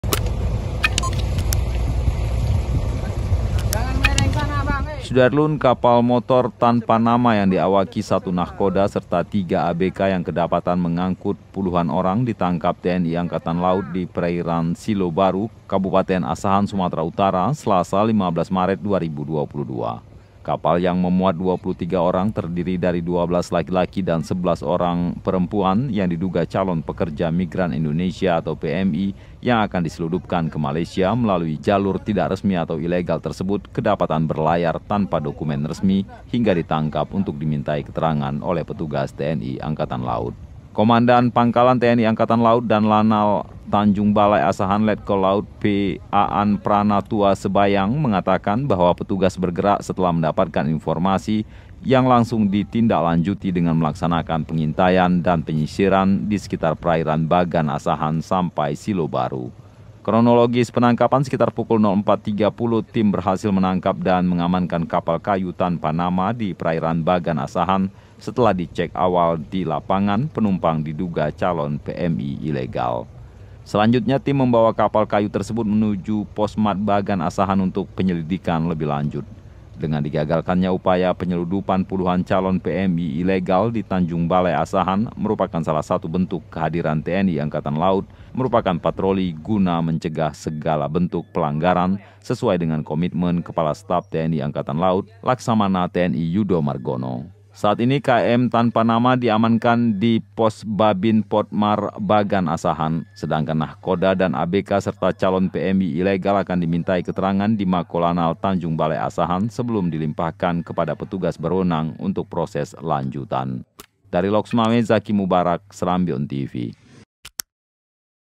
Lagi, TNI AL Gagalkan Puluhan Calon PMI Ilegal di TBA
Laporan Zaki Mubarak | Lhokseumawe
SERAMBINEWS.COM – Kapal Motor (KM) tanpa nama yang diawaki satu Nakhoda serta tiga ABK yang kedapatan mengangkut puluhan orang ditangkap TNI Angkatan Laut di Perairan Silo Baru, Kabupaten Asahan, Sumut, Selasa (15/3/2022).
Kapal yang memuat 23 orang, terdiri dari 12 Laki-laki dan 11 perempuan yang diduga calon Pekerja Migran Indonesia (PMI) yang akan diselundupkan ke Malaysia melalui jalur tidak resmi (ilegal) itu kedapatan berlayar tanpa dokumen resmi hingga ditangkap untuk dimintai keterangan oleh petugas TNI AL.
Komandan Pangkalan TNI Angkatan Laut (Danlanal) Tanjung Balai Asahan Letkol Laut (P) Aan Prana Tua Sebayang mengatakan bahwa petugas bergerak setelah mendapatkan informasi yang langsung ditindaklanjuti dengan melaksanakan pengintaian dan penyisiran di sekitar perairan Bagan Asahan sampai Silo Baru.
Kronologis penangkapan sekitar Pukul 04.30  tim berhasil menangkap dan mengamankan kapal kayu tanpa nama di Perairan Bagan Asahan, setelah dicek awal di lapangan, penumpang diduga calon PMI Ilegal.
Selanjutnya tim membawa kapal kayu tersebut menuju Posmat Bagan Asahan untuk penyelidikan lebih lanjut.
Dengan digagalkannya upaya penyelundupan puluhan calon PMI ilegal di Tanjung Balai Asahan merupakan salah satu bentuk kehadiran TNI Angkatan Laut melakukan Patroli guna mencegah segala bentuk pelanggaran sesuai dengan komitmen Kepala Staf TNI Angkatan Laut, Laksamana TNI Yudo Margono.
Saat ini KM Tanpa Nama diamankan di Posbabinpotmar Bagan Asahan.

Sedangkan Nakhoda dan ABK serta Calon PMI ilegal akan dimintai keterangan di Mako Lanal Tanjung Balai Asahan sebelum dilimpahkan kepada petugas berwenang untuk proses lanjutan.(*)